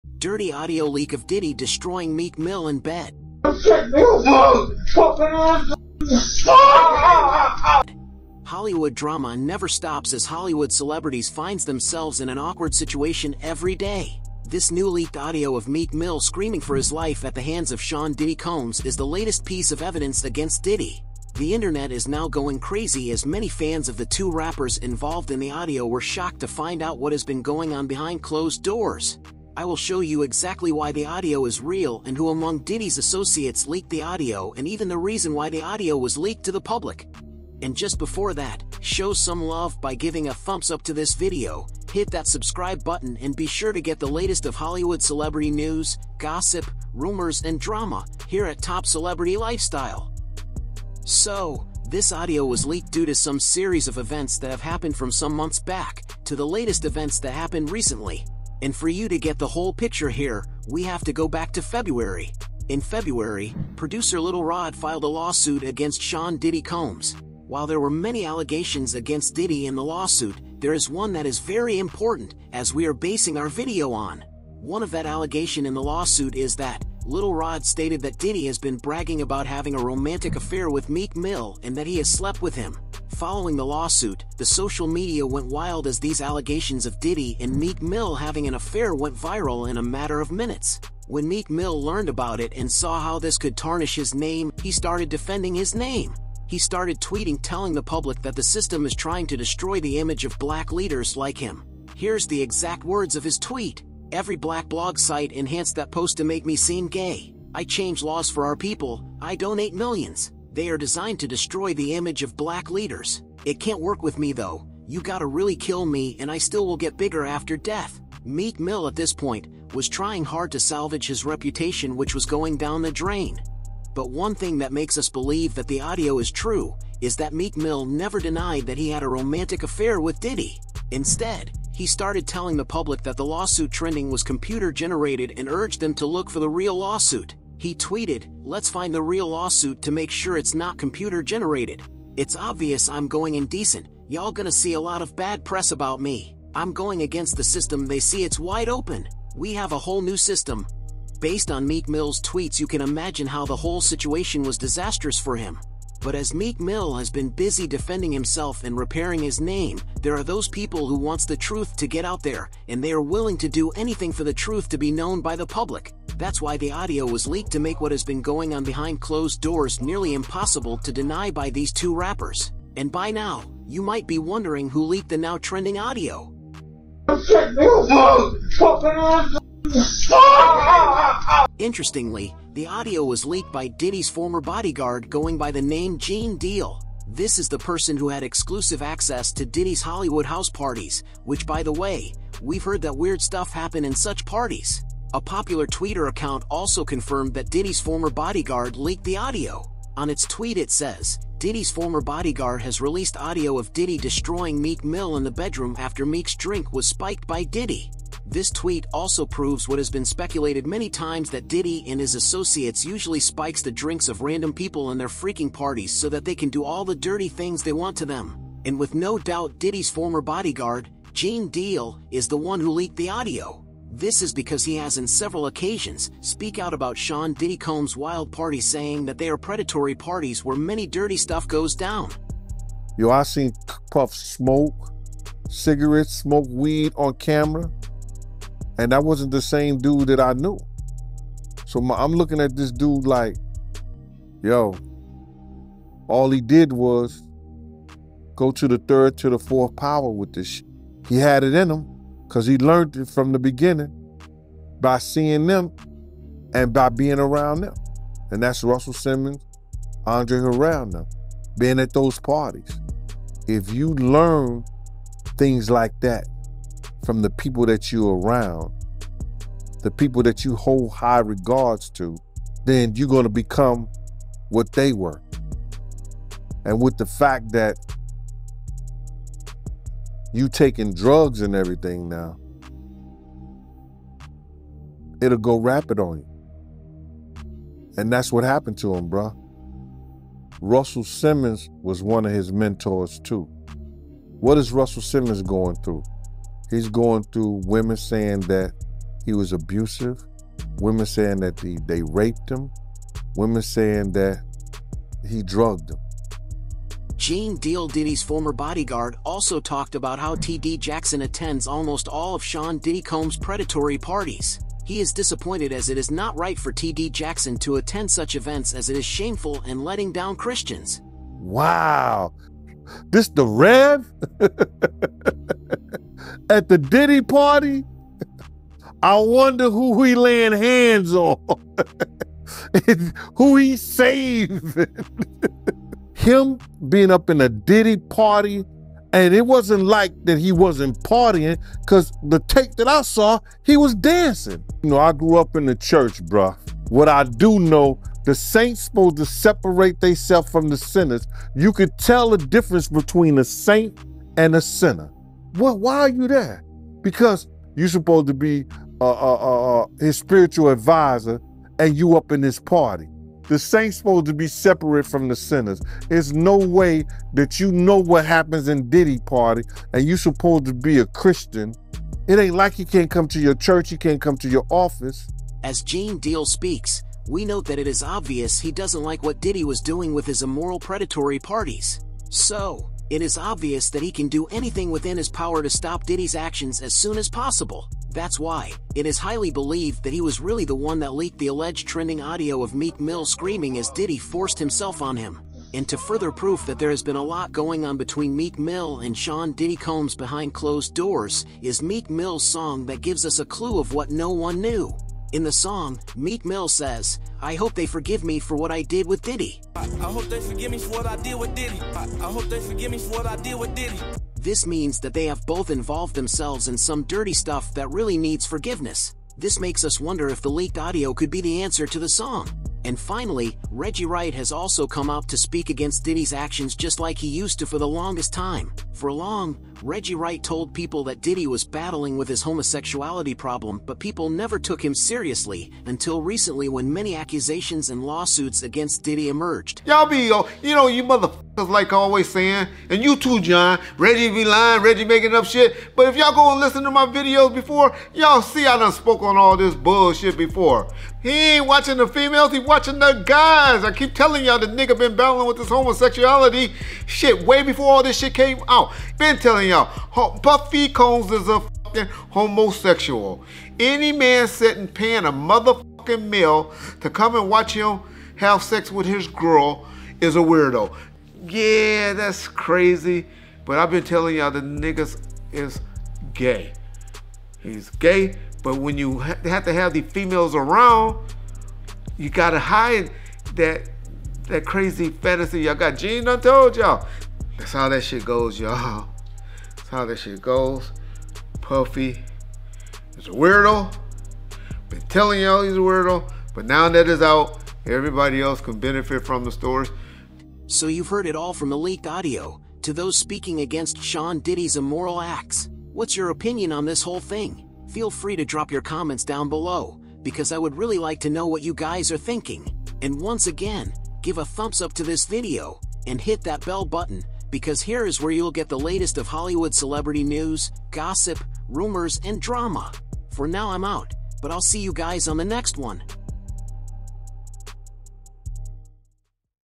Dirty Audio Leak!!! Of Diddy Destroying Meek Mill in Bed | Hollywood Drama getting Messy
This video will highlight on the Dirty Audio Leak of Diddy Destroying Meek Mill in Bed. You will see how Hollywood drama is becoming much as Hollywood celebrities get involved in yet another scandal. This leaked audio of Meek Mill and Sean Diddy Combs, is a new piece of evidence against Diddy apart from the many lawsuits against. For example, Little Rod’s lawsuit filed in January, stated that Diddy was having a romantic affair with Meek Mill. These lawsuits against Diddy, supports the widespread allegations that Diddy regularly holds Hollywood house parties which are always dirty and predatory in nature. You will also see how Gene Deal Diddy’s former bodyguard and Reggie Wright have spoken exposing Sean Diddy Combs for who he really is.
Enjoy this video and other similar videos here at Top Celebrity Lifestyle channel.